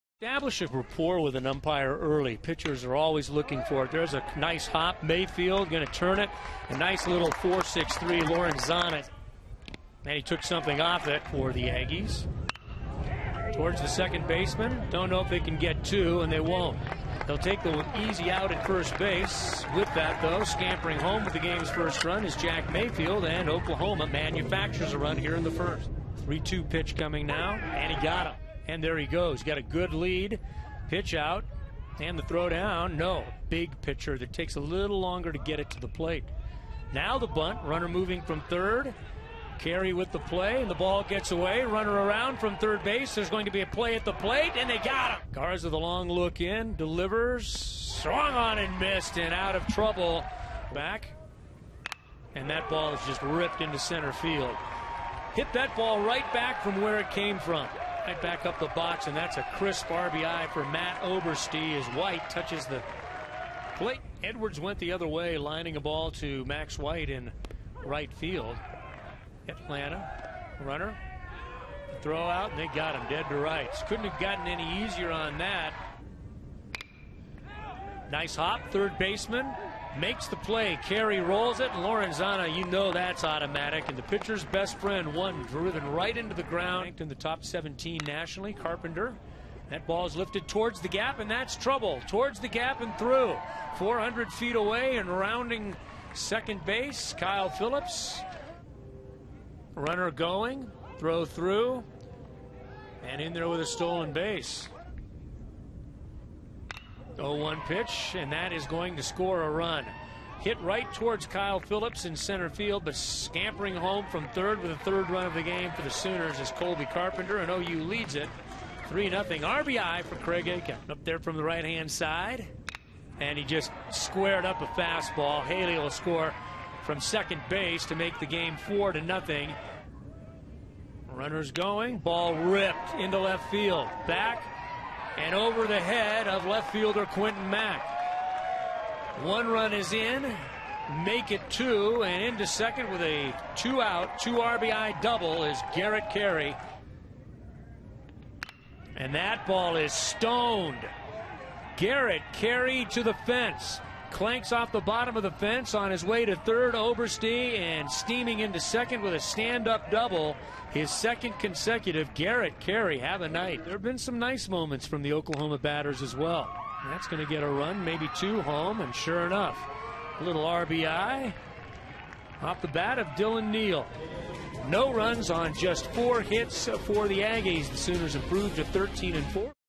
OU 8, New Mexico State 0 Highlights
No. 17 Oklahoma opened a 10-game homestand with an 8-0 shutout of the Aggies Tuesday in Norman.